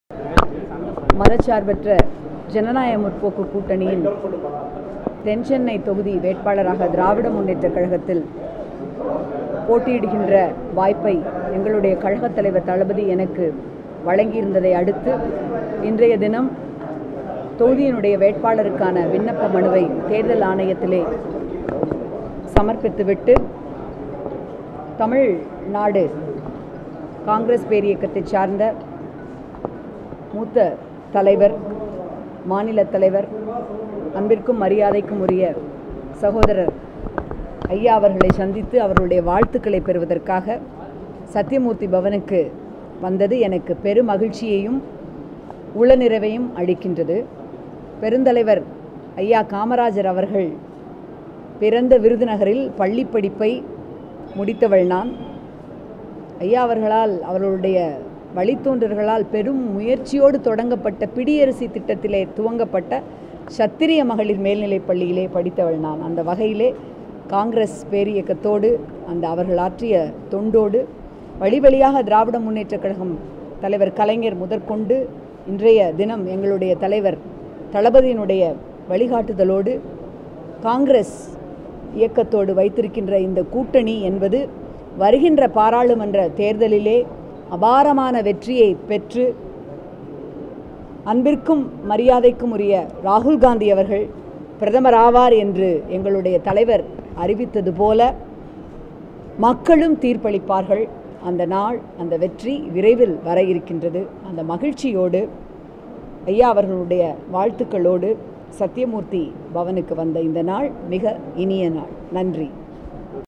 DMK South Chennai candidate Thamizhachi Thangapandian TNCC President KS Azhagiri
DMK South Chennai candidate Thamizhachi Thangapandian meets TNCC President KS Azhagiri and seeks Congress support